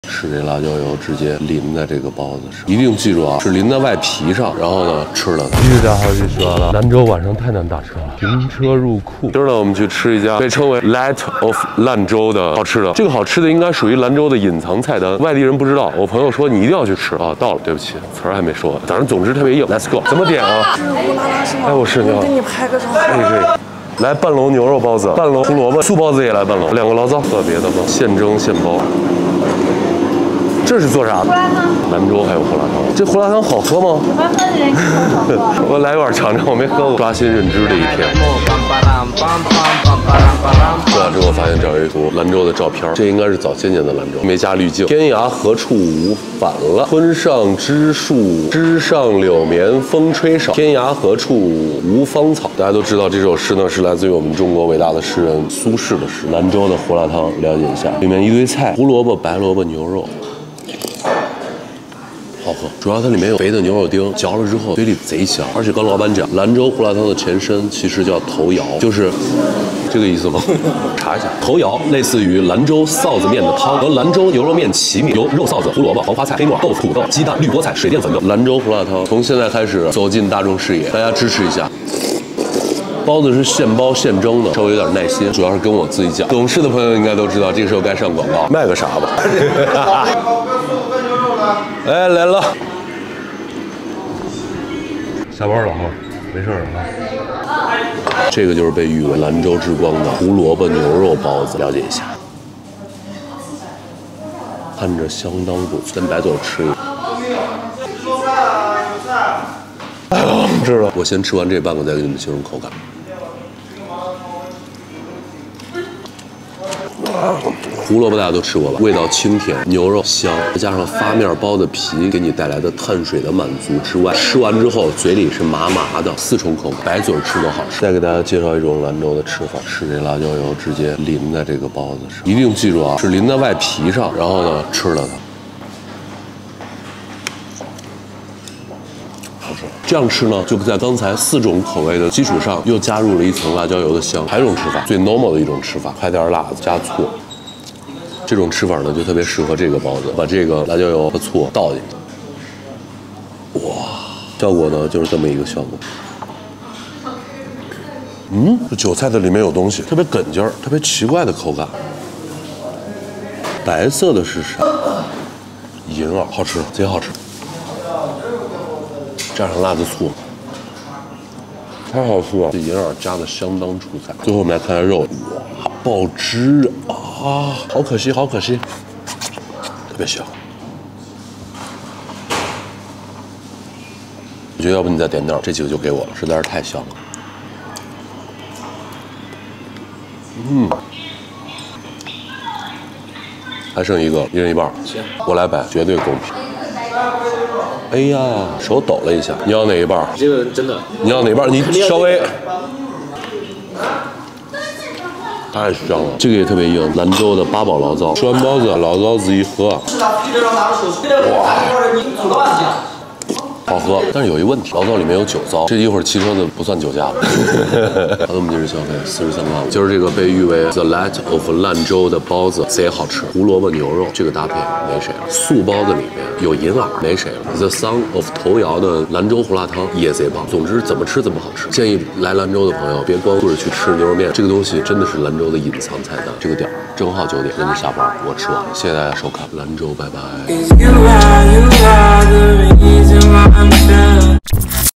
【乌啦啦vlog】兰州正宗胡辣汤+包子，纯手工制馅揉面，淋上辣椒油，堪称一绝！| 特别乌啦啦
这次带大家来吃号称“兰州之光”的隐藏菜单，牛肉胡萝卜包子，纯手工制馅揉面，一笼七个，太香了！

U, 大家好，这里是乌啦啦！
我会带大家体验中国美食之旅，
展现人间烟火里的真实味道。
Let's go!